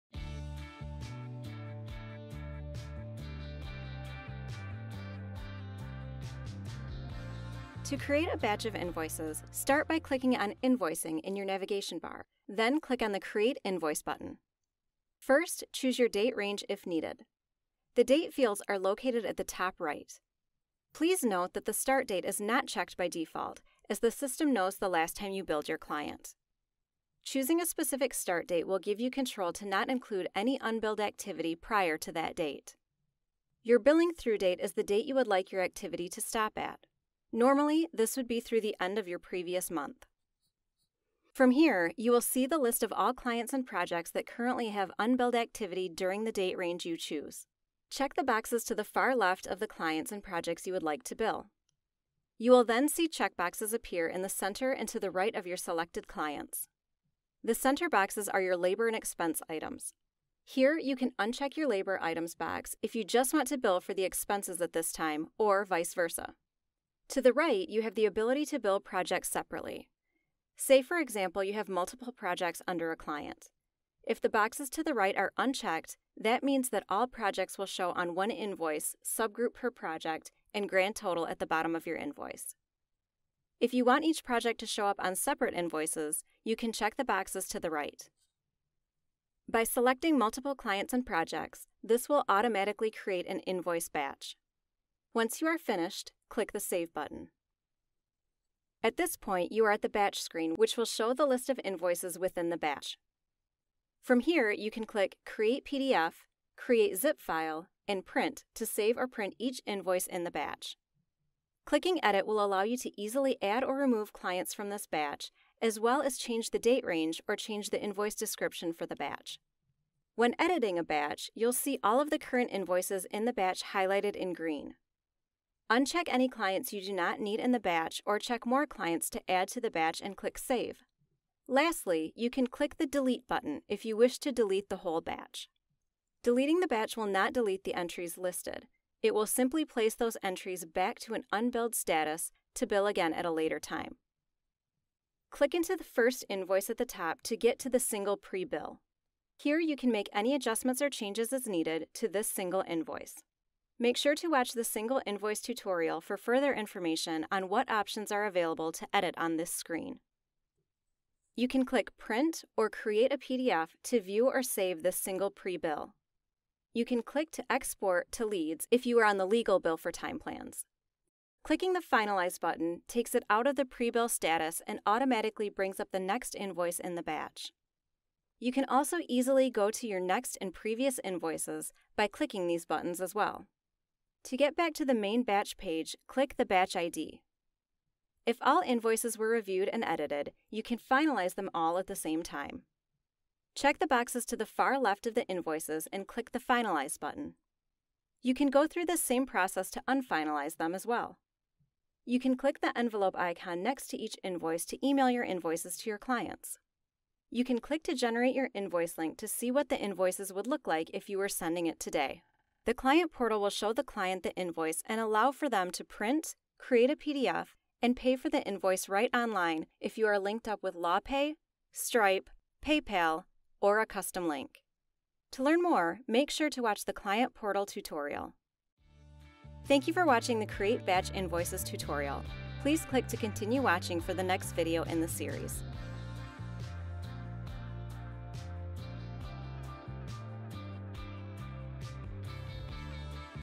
Creating Batch Invoices | Bill4Time - Invoicing
Save time with the end of the month or project with this powerful feature. Learn how to create a batch of invoices in this Bill4Time tutorial.